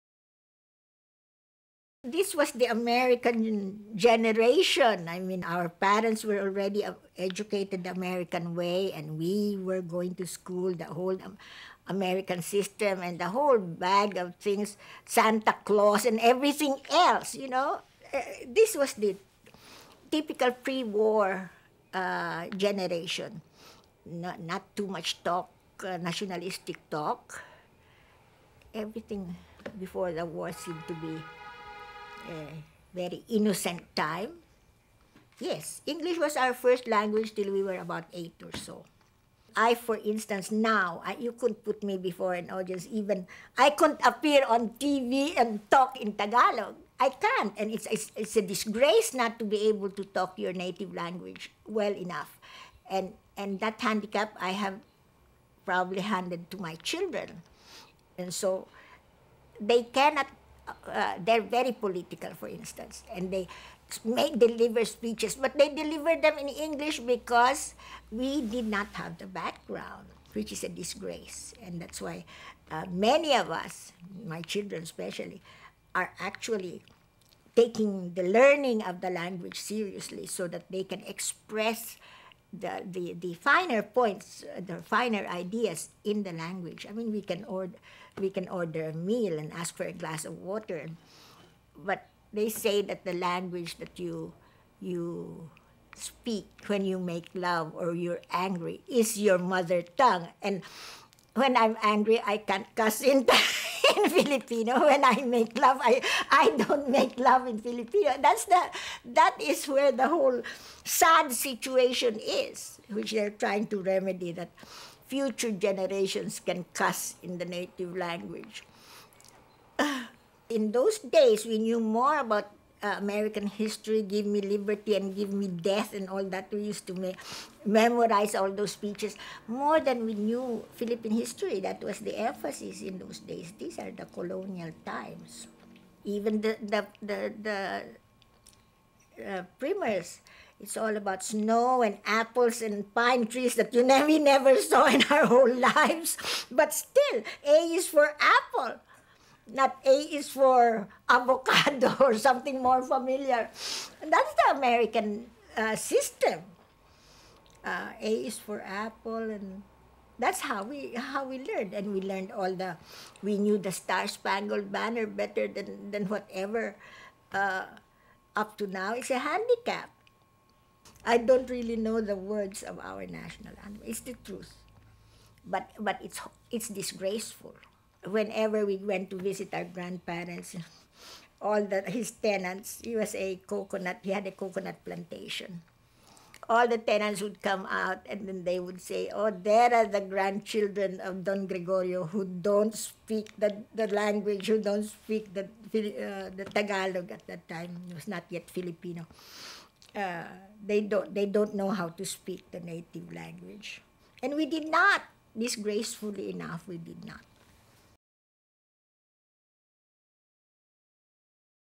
Virginia Benitez Licuanan & English.mov
Virginia Benitez Licuanan describes the effect on her of the American school system in the Philippines-- this was before World War II.  The interview was done in Manila, 1986.  Three and a half minutes.